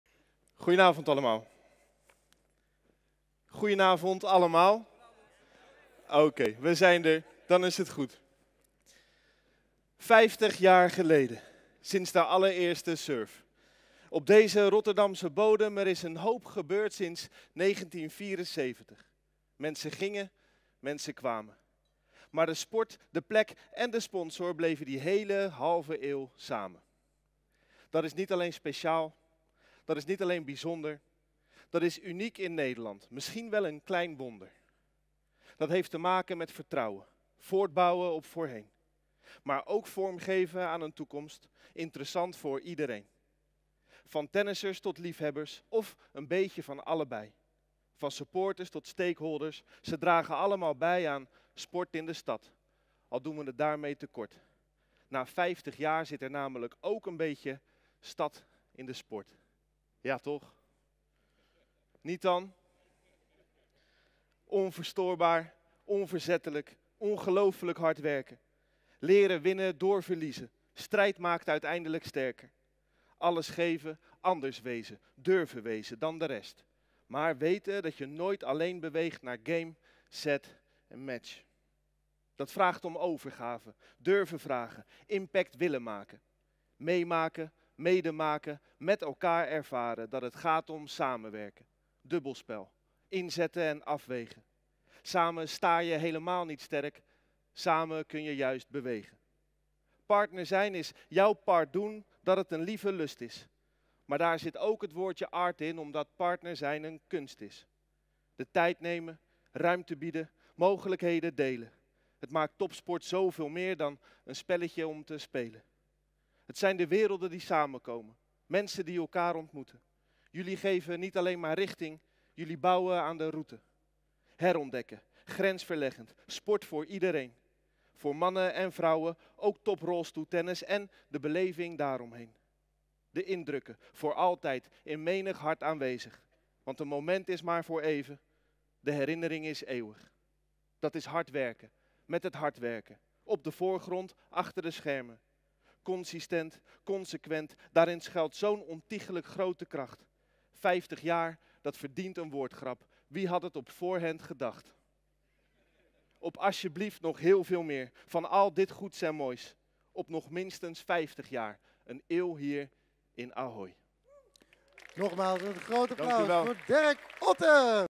Rotterdamse stadsdichter Derek Otte brengt een ode aan ABN AMRO Open
Geen beter moment voor een speciale voordracht, dan het 50-jarige jubileum van ABN AMRO Open. Dank aan Derek Otte!